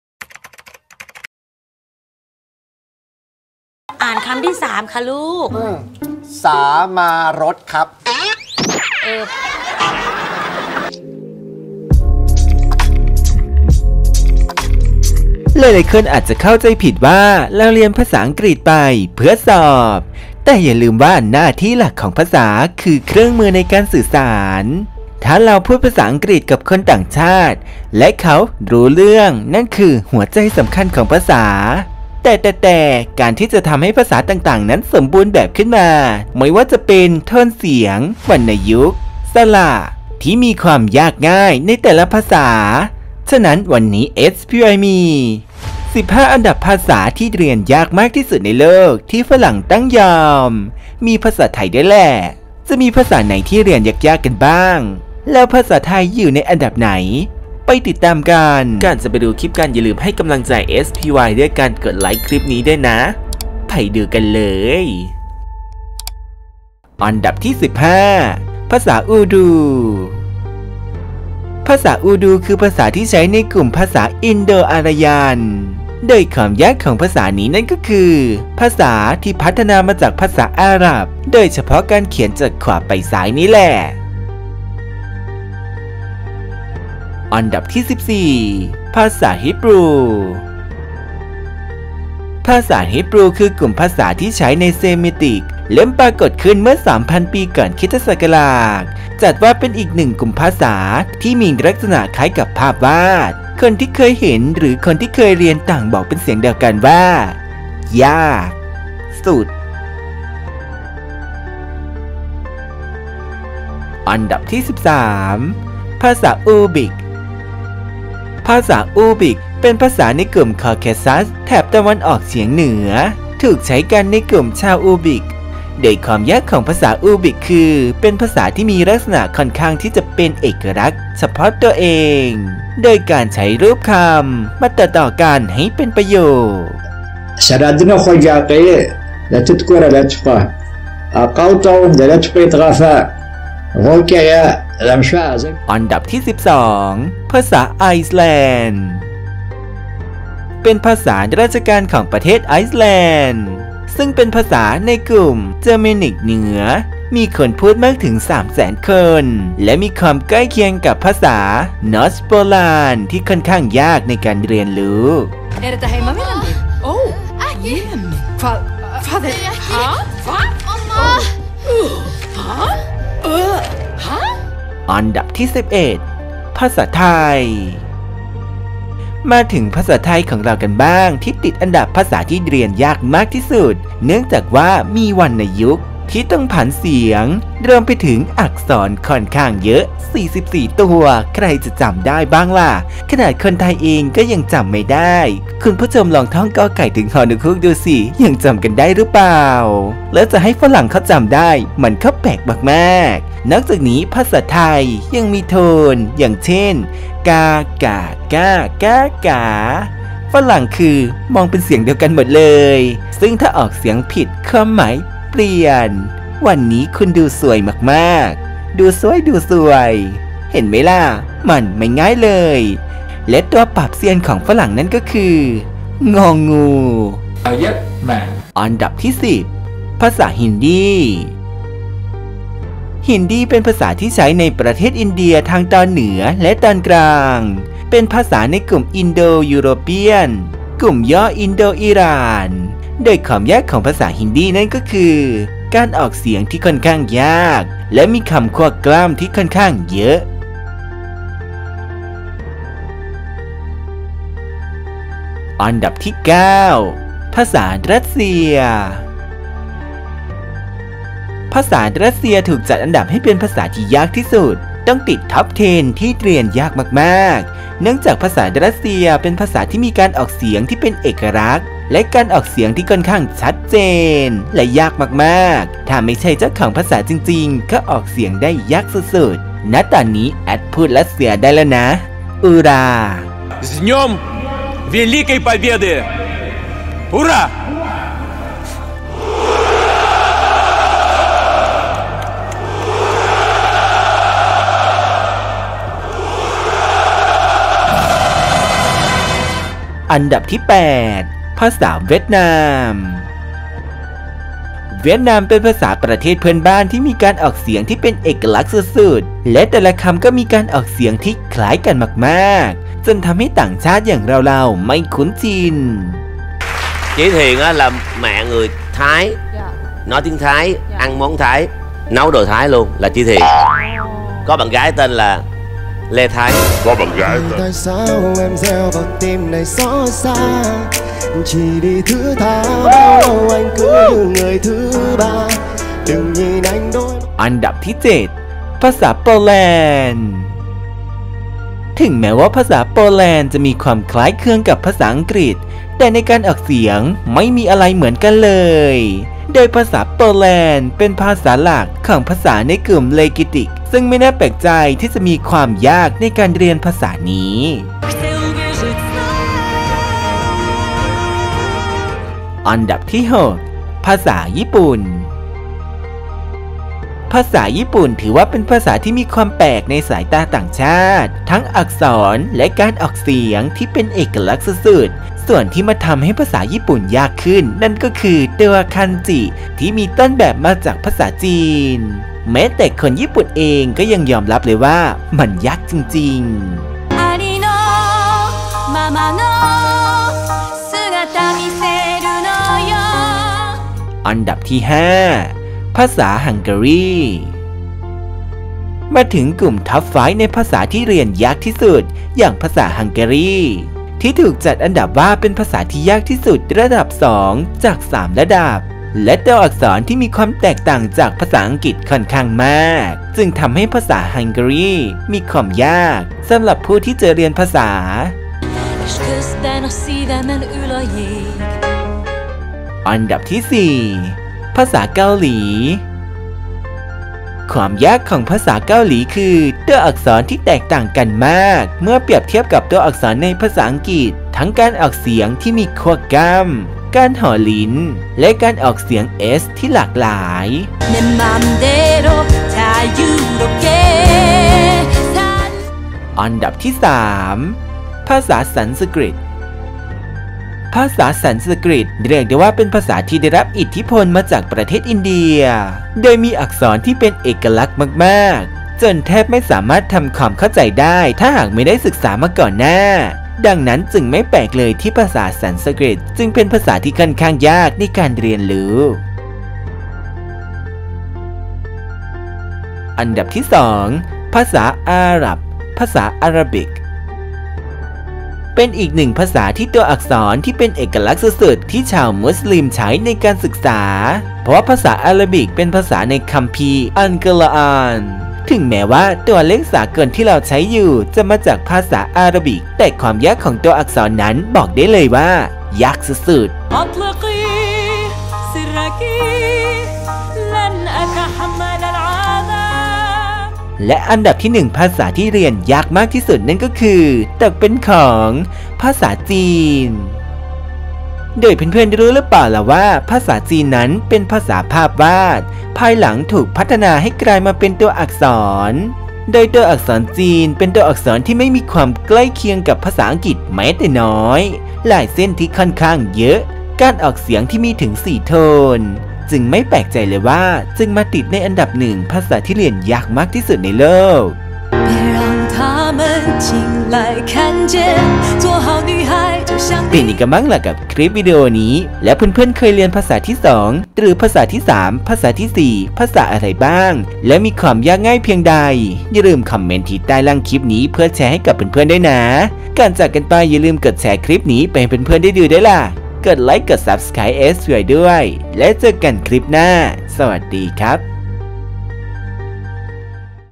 15 อันดับ ภาษาสุดโหด!! เรียนยาก ฝรั่งต้องยอม(มีไทย)
ภาษาคือสิ่งที่ทำให้เราสามารถสื่อสารกับคนต่างชาติได้มากขึ้น แต่ถึงอย่างนั้นแต่ละภาษาก็มีเอกลักษณ์ของความยาก-ง่ายในการเรียนแตกต่างกัน